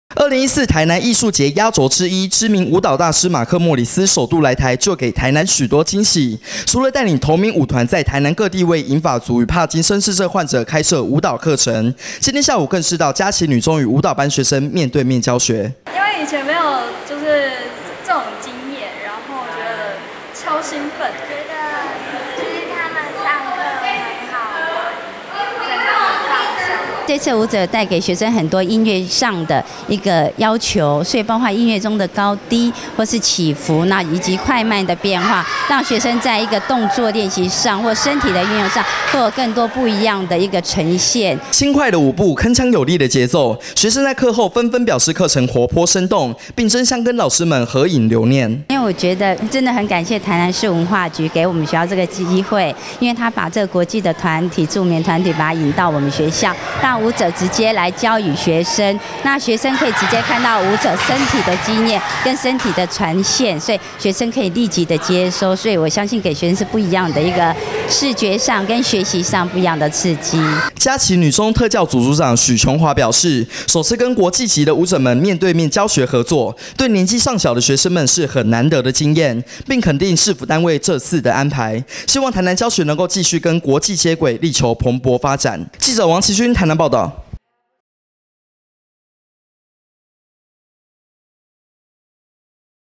國際舞團家齊開課交流 多元推廣舞蹈教育 今日大話新聞
【今日大話新聞記者/王麒勳/台南報導】　2014台南藝術節壓軸之一，知名舞蹈大師馬克‧莫里斯(Mark Morris)首度來台就帶給台南許多驚喜，除了帶領同名舞團在台南各地為銀髮族與帕金森氏症患者開設舞蹈課程，今天下午更是到家齊女中與舞蹈班學生面對面教學。